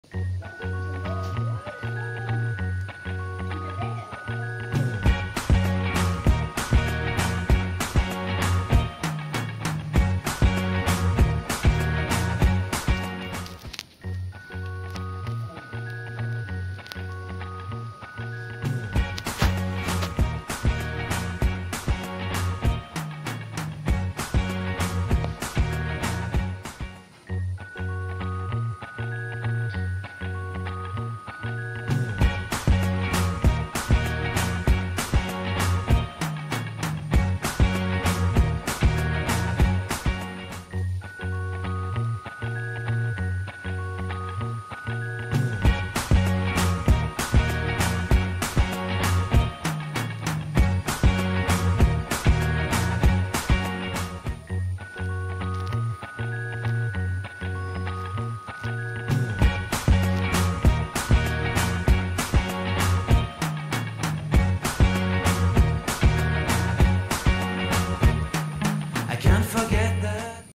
Cat Family - Cleaning Time for Kitties!! 💖
The adorable kittens busy washing themselves.. Mama has certainly taught them well! 💖